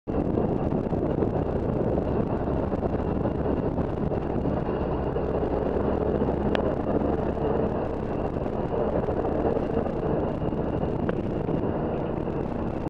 Inicio del descenso hacia Autisha
Inicio del camino de bajada hacia Autisha, Zona Natural.